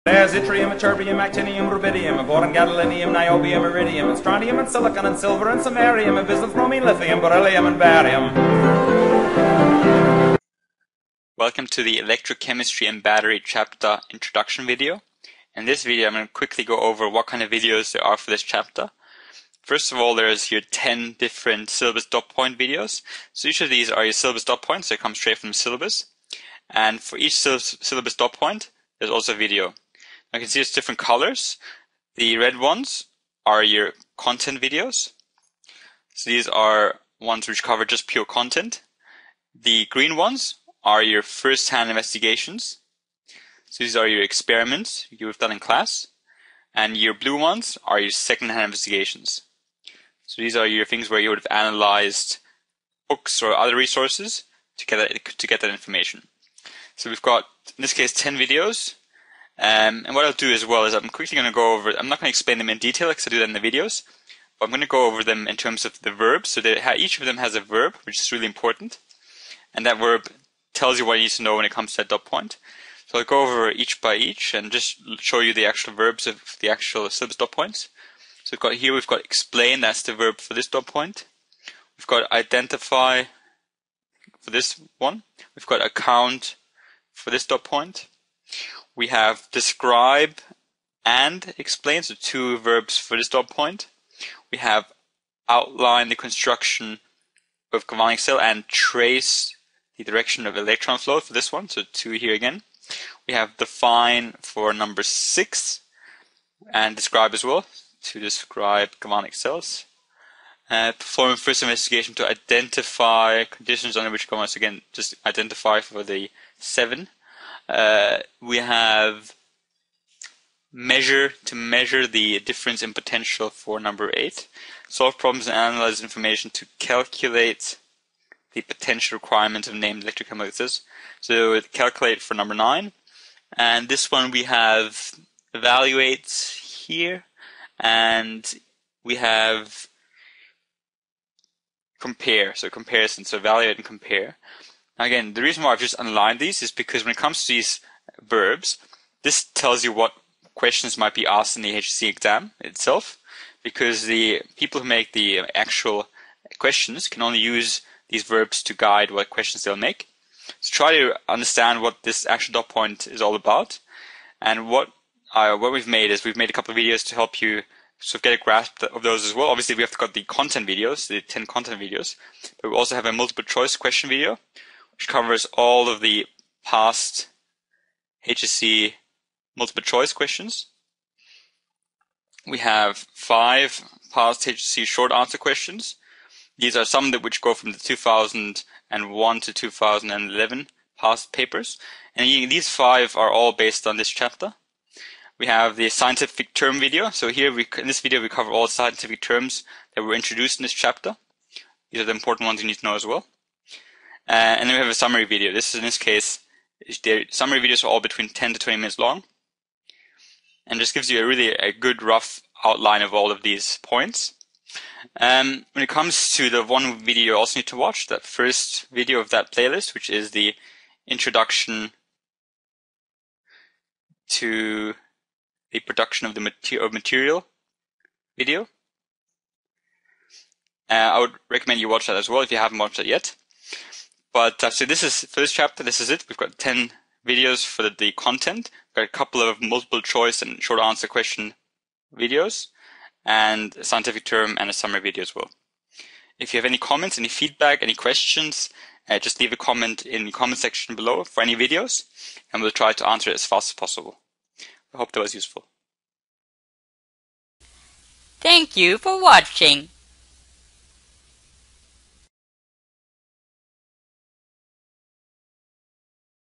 Introduction to chapter (electrochemistry)
Gives a brief introduction to the videos that cover the electochemistry chapter of the production of material module